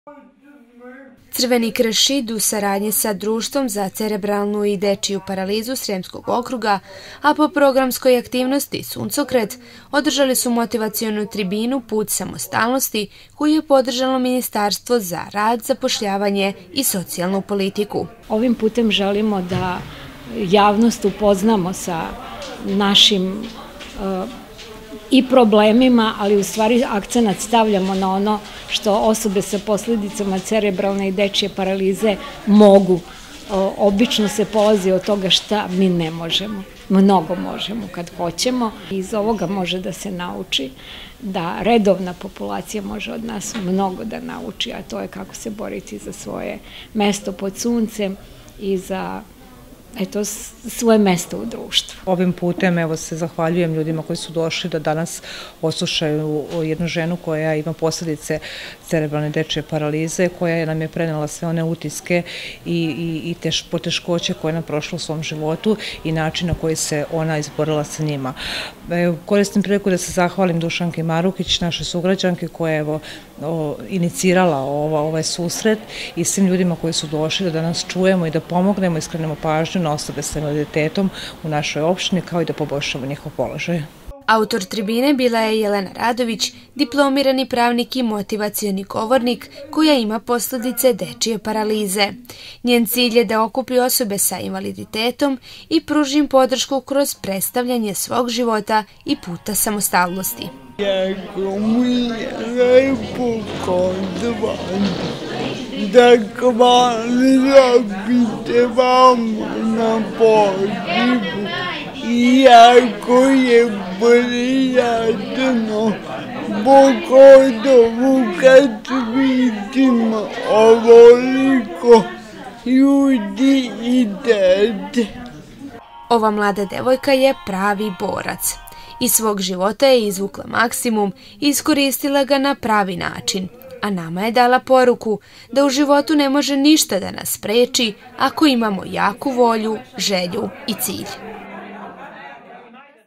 Crveni krst radionica
U Crvenom Krstu u Šidu održana je tribina pod nazivom “Put samostalnosti” čija je autorka bila Jelena Radović, diplomirani pravnik i motivacioni govornik. Cilj trbine bio je pružanje podrške osobama sa invaliditetom.